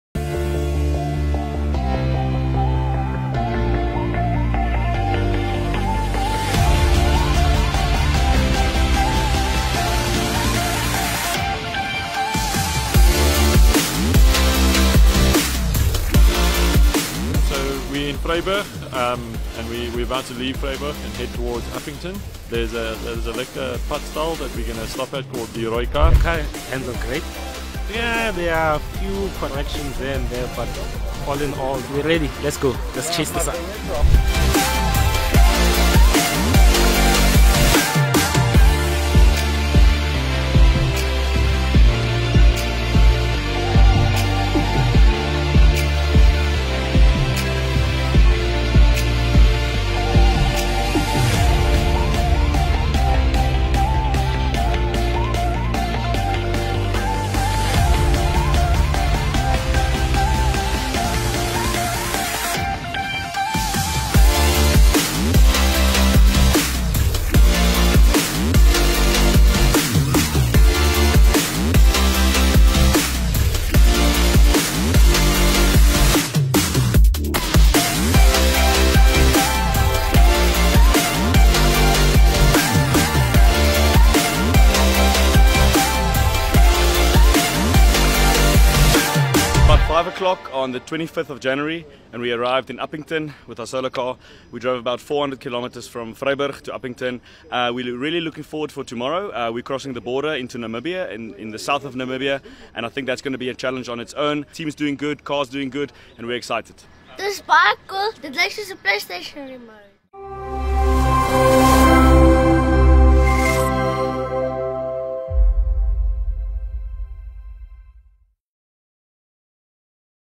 The Sunchaser 4, a solar car built by students is currently touring Namibia
The Sunchaser 4, a fourth generation solar car built by students and staff from Tshwane University of Technology, is currently touring Namibia.